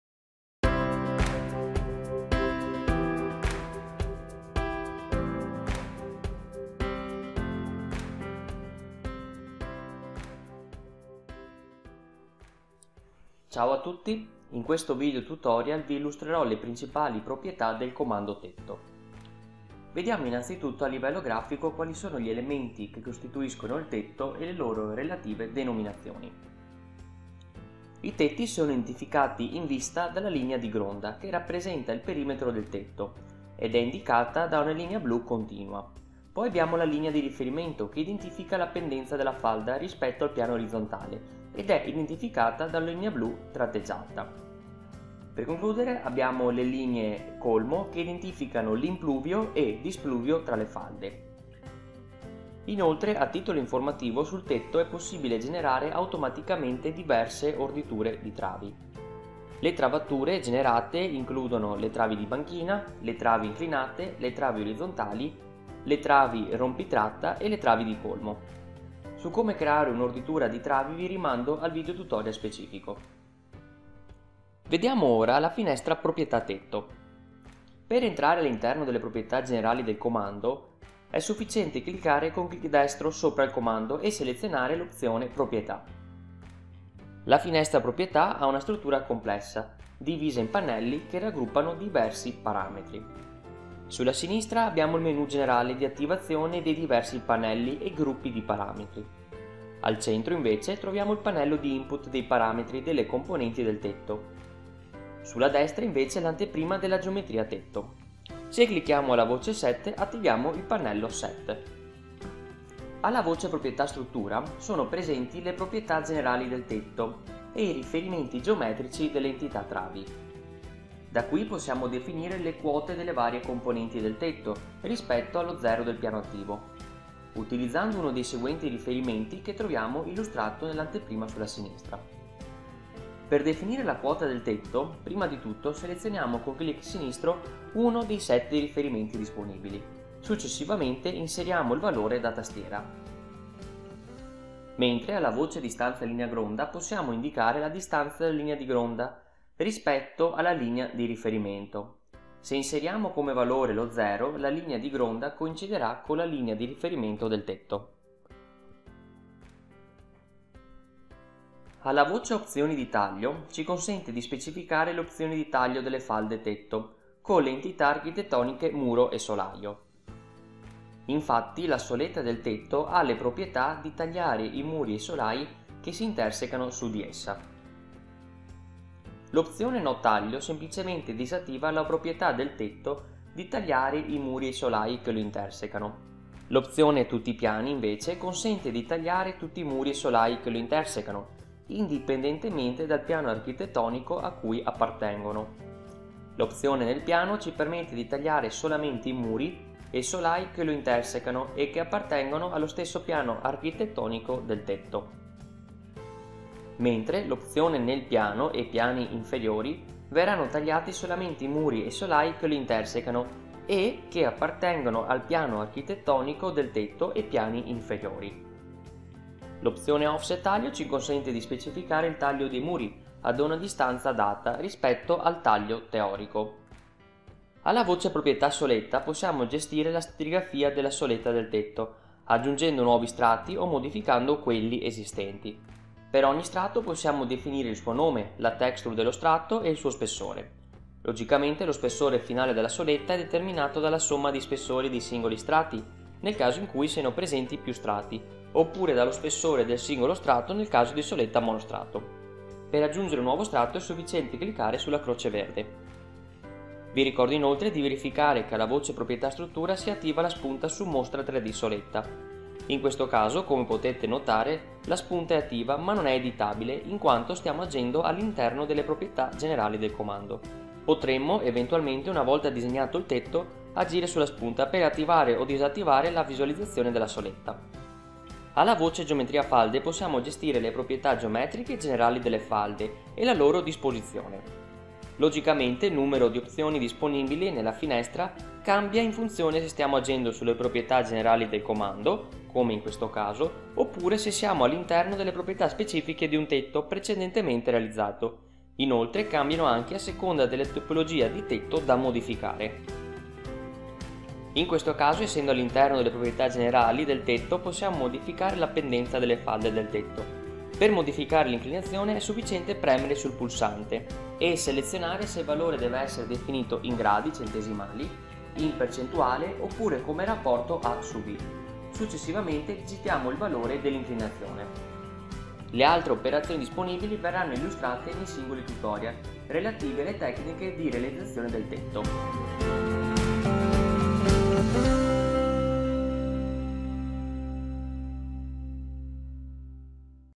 OPZIONI TETTO
In questo video tutorial vengono illustrate le proprietà generali del comando tetto